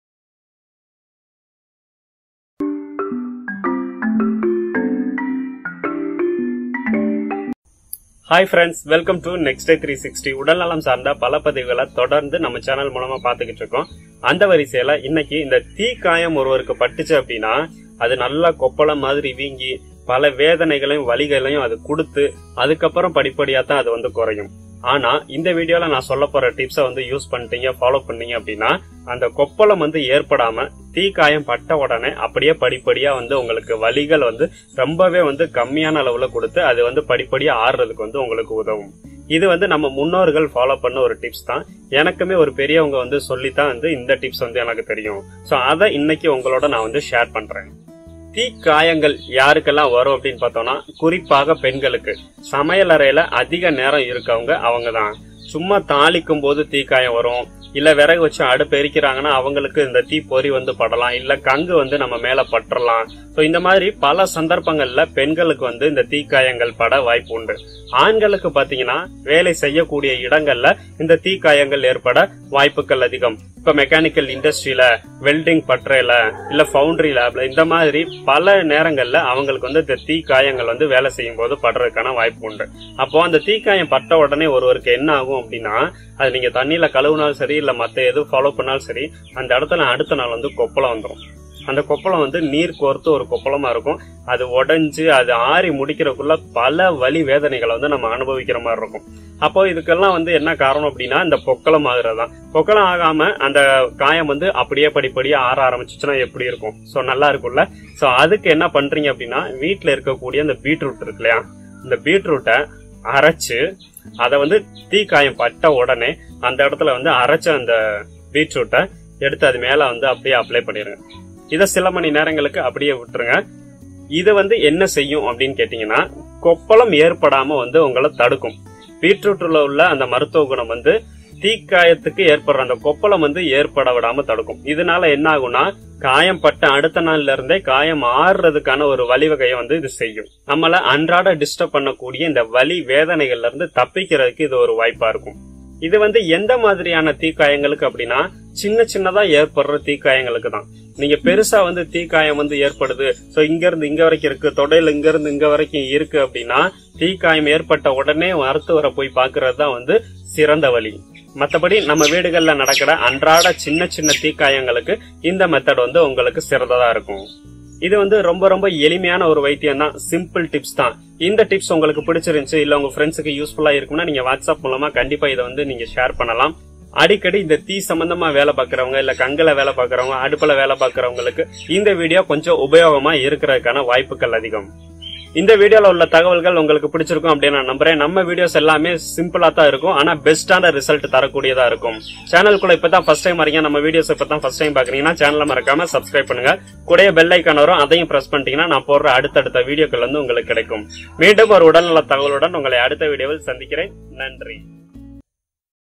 தீக்காயத்தினால் கொப்பளம் ஏற்படாமல் தடுக்க பாட்டி வைத்தியம் | Home remedy for minor Burns
தீக்காயத்தினால் நாம் பலரும் பலவிதமான வலிகளையும் வேதனைகளையும் அனுபவித்திருப்போம் அதற்குக் காரணம் அந்த தீக்காயத்தின் மேல் ஏற்படும் கொப்பளம் தான்.

இந்தக் கொப்பளம் தான் நமக்கு வலிகளை  மேலும் அதிகப்படுத்தும்.
 இந்த காணொளியில் நாம் காணவிருப்பது என்னவென்றால் அப்படி தீக்காயம் நமக்கு ஏற்பட்டு விட்டால் அந்தக் கொப்பளம் ஏற்படாமல் தவிர்ப்பதற்கான இயற்கை வழிமுறை தான். உங்களுக்கு காயம் ஏற்பட்ட உடனே நீங்கள் இதனை பின்பற்றினால் கொப்பளம் ஏற்படாமல் முற்றிலும் தடுக்க முடியும். வீட்டில் நடக்கக்கூடிய சின்ன சின்ன தீக்காயங்களுக்கு இதனை பயன்படுத்துவது மிகவும் எளிது. முழுமையாக தெரிந்து கொள்ள காணொலியை பாருங்கள் பயன் பெறுங்கள்